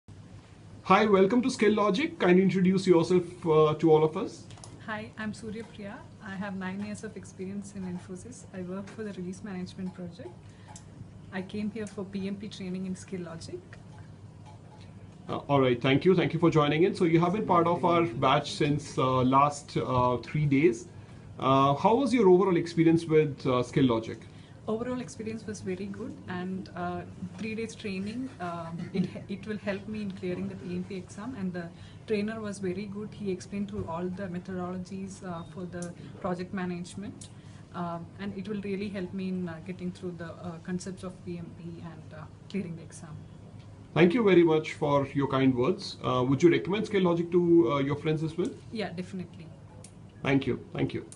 SuryaPriya Review on Skillogic PMP® Training
SuryaPriya who works for INFOSYS, attended Skillogic PMP® training at Bangalore / Bengaluru. Here is her review about training.